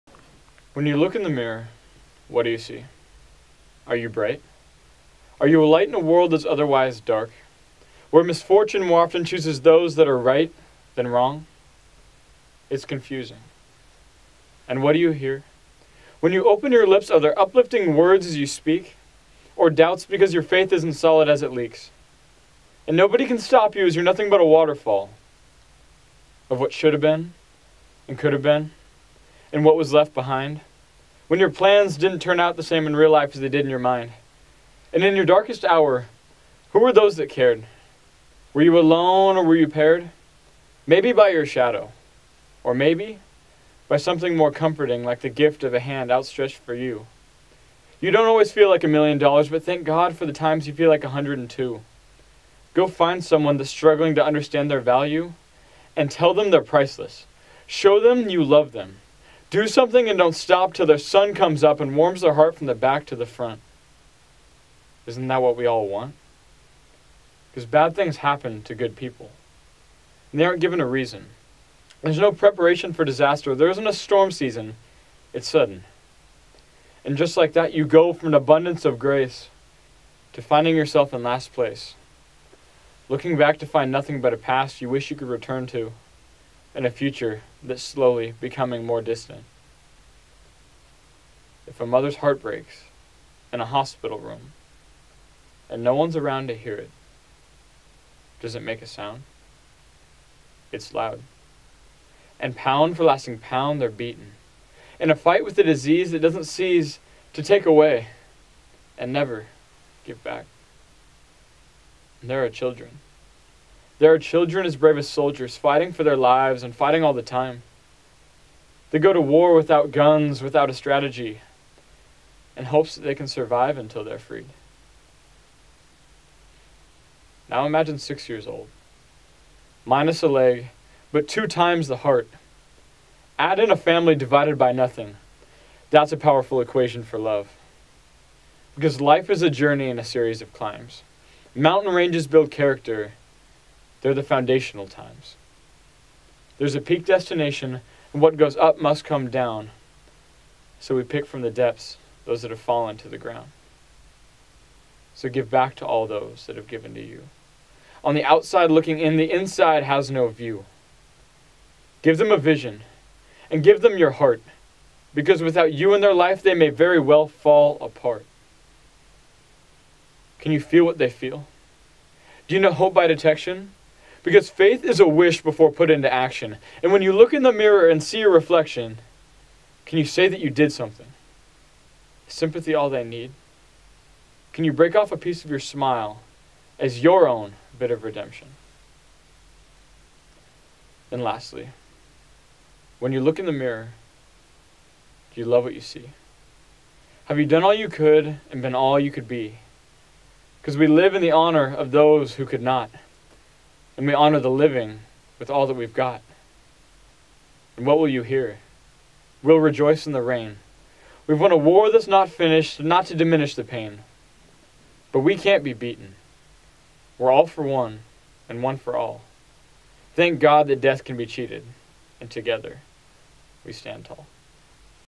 Joshua King - When you look in the mirror
A Poet from the Biola Spoken Word Team